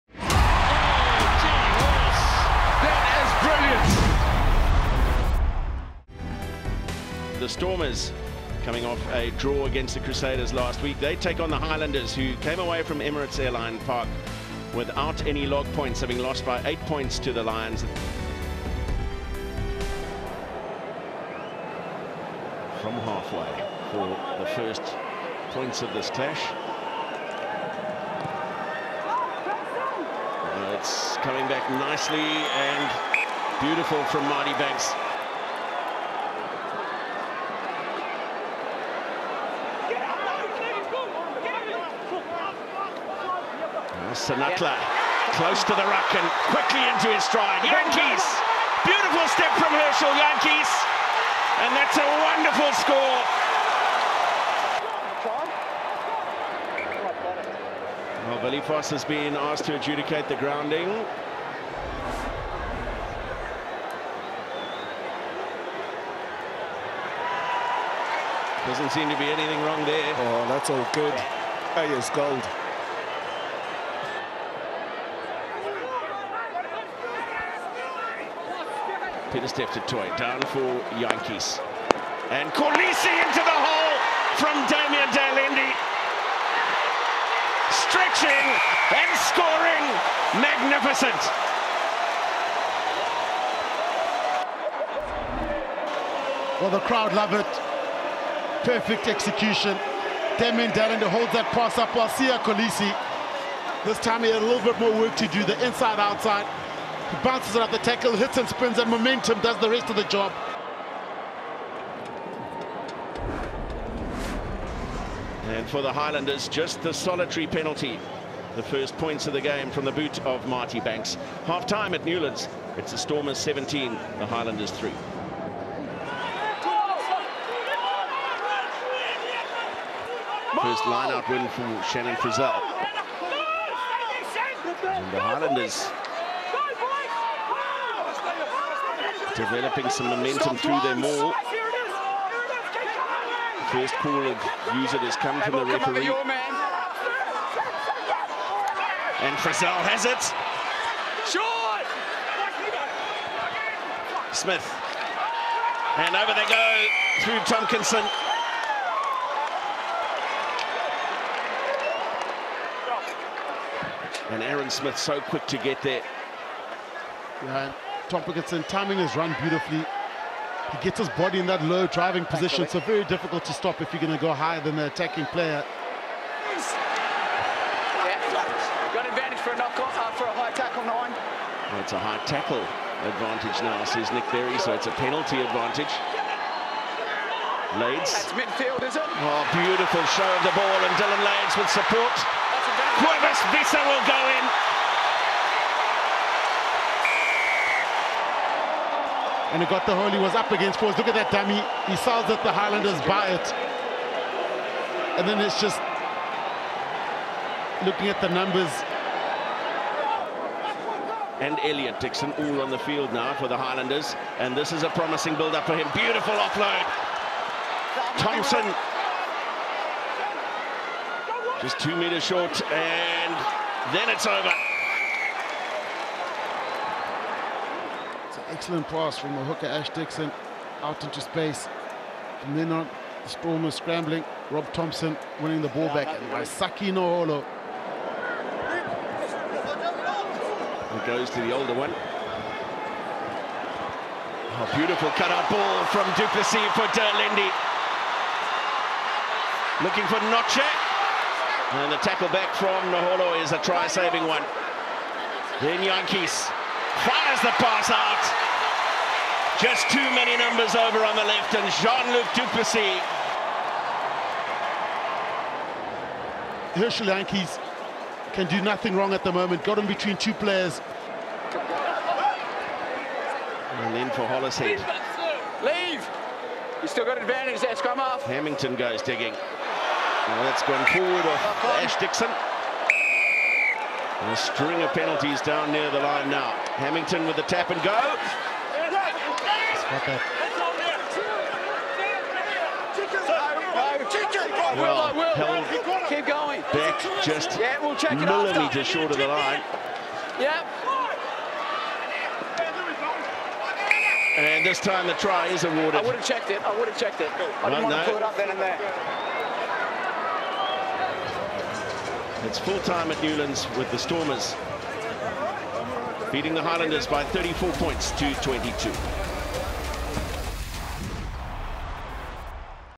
Stormers v Highlanders | Super Rugby 2019 Rd 15 Highlights
The Stormers secure a solid 34-22 home victory in Cape Town, the Highlanders scoring a last minute try in a gutsy performance to the end.